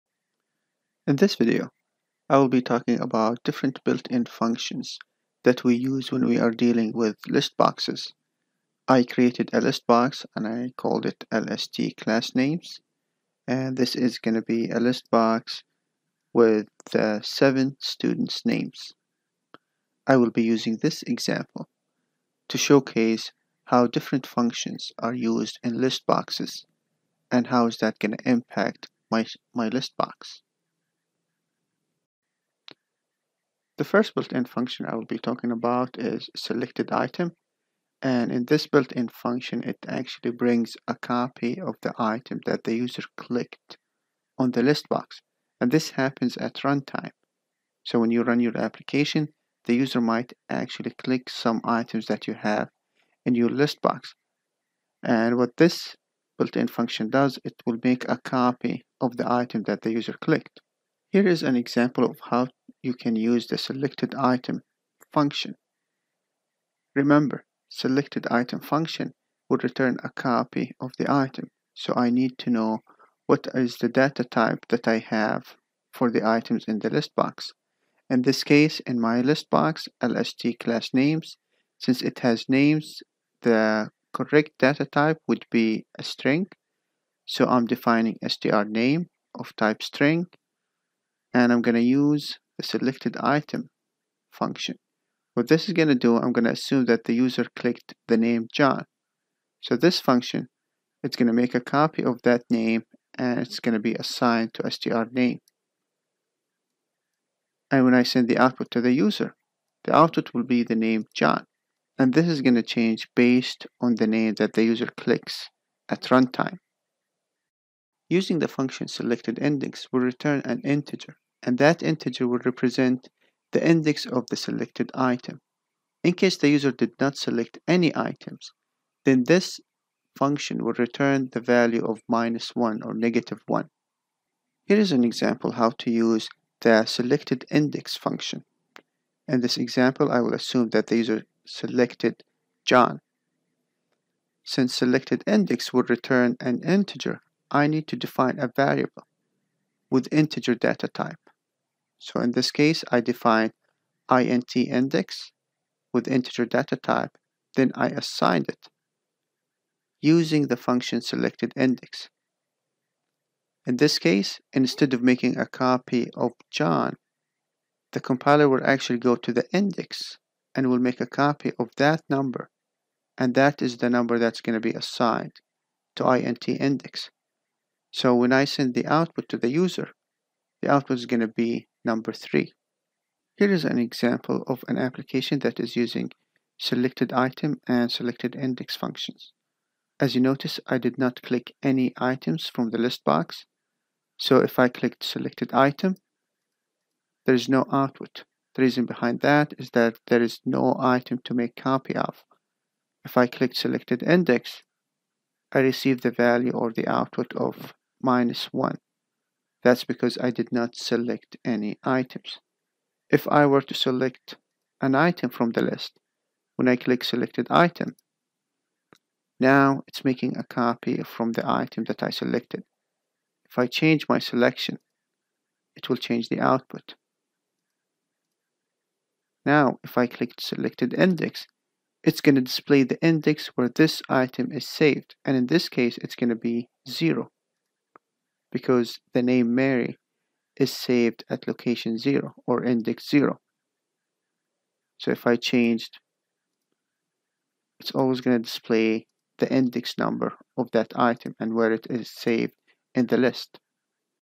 ListBox Selected Item and Selected Index Functions
How to use Selected items and selected index functions in ListBox.